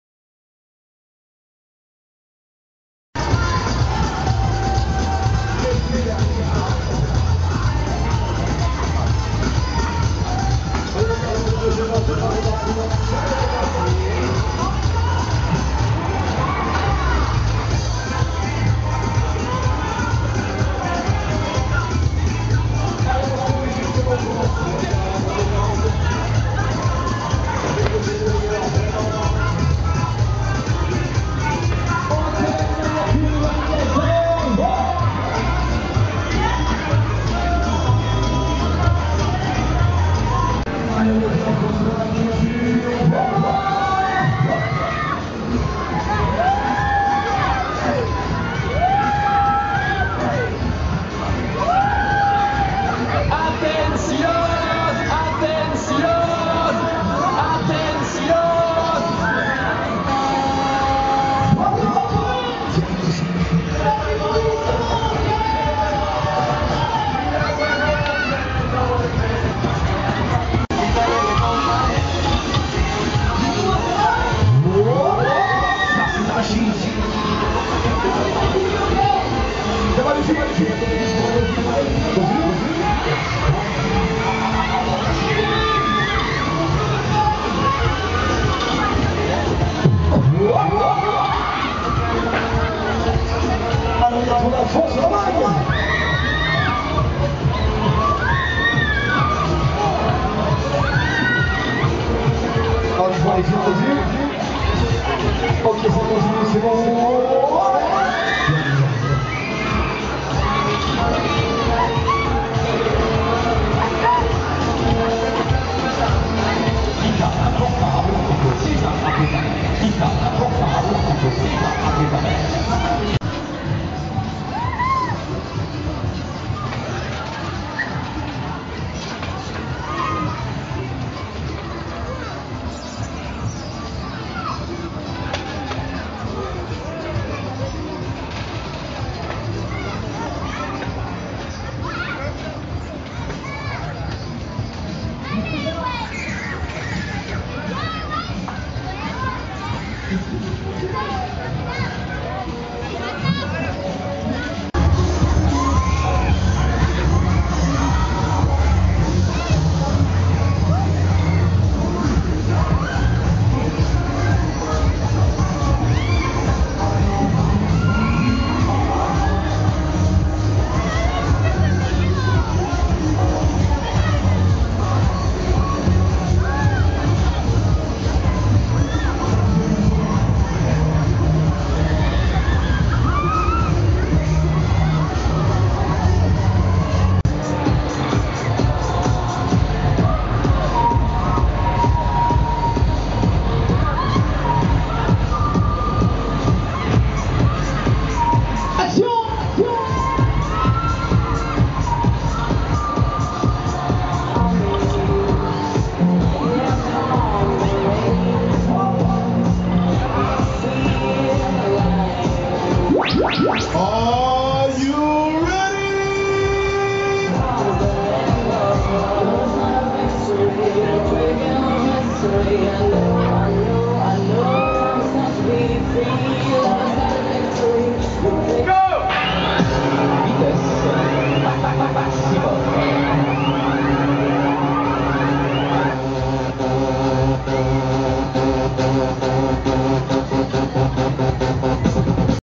Fête foraine de Massy 2016
La fête foraine de Massy (91) le dimanche 5 juin 2016.